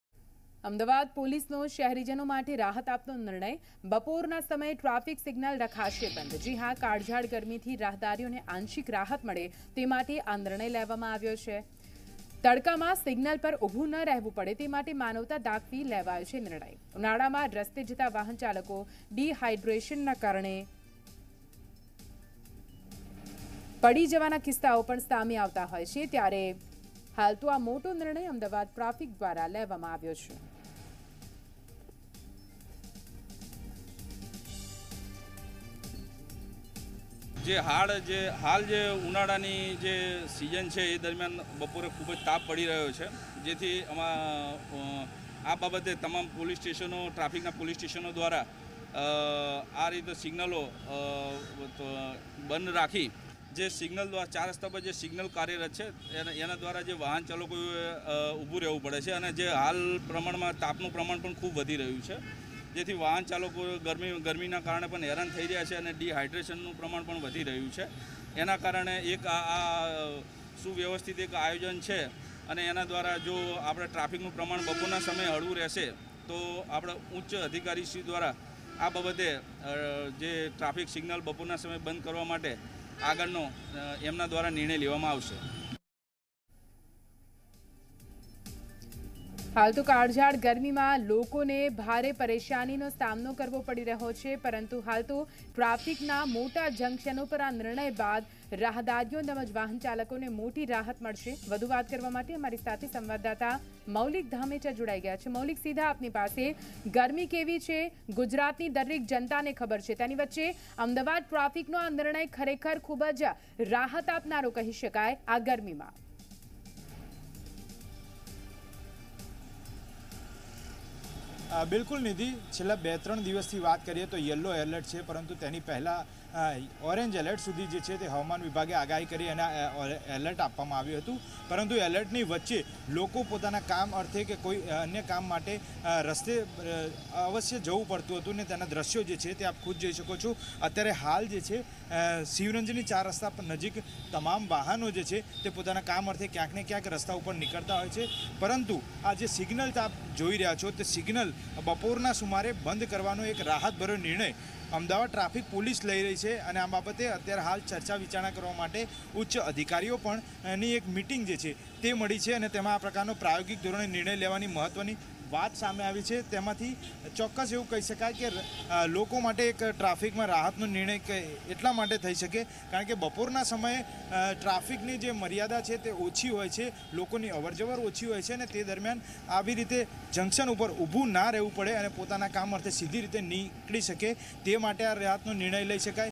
Traffic signals in Ahmedabad to remain shut during noon hours due to blistering heat | Zee News
અમદાવાદ પોલીસનો શહેરીજનો માટે રાહત આપતો નિર્ણય---

બપોરાના સમયે ટ્રાફિક સિગ્નલ રખાશે બંધ---

કાળઝાળ ગરમીથી મળશે રાહદારીઓને આંશિક રાહત--

ટ્રાફિકના મોટા જંક્શનો પર બપોરે સિગ્નલ બંધ રહેશે--

તડકામાં સિગ્નલ પર ઉભુ ના રહેવું પડે તે માટે માનવતા દાખવી લેવાયો નિર્ણય--

ઉનાળામાં રસ્તે  જતા વાહન ચાલકો ડીહાઇડ્રેશનને કારણે પડી જવાના કિસ્સાઓમાં સામે આવતા લેવાયો નિર્ણય